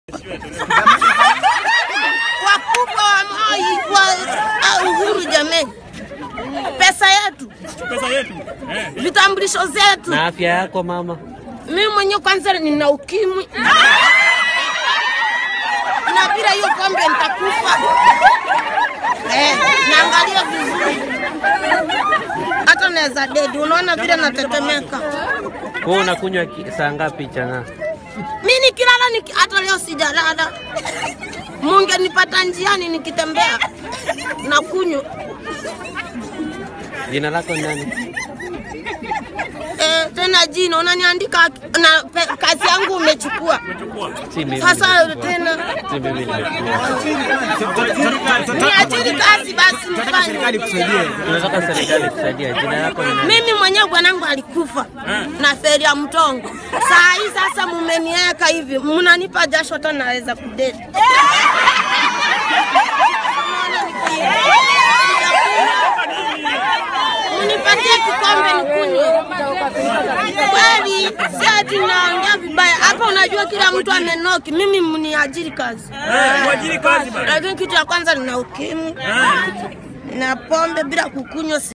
Mombasa brewer urges state to provide alternative source of income
The woman, Susan Chacha has called on the state to provide other means of earning a living for the brewers in the wake of a directive by President Uhuru Kenyatta, calling for a nation-wide crackdown on illicit brew.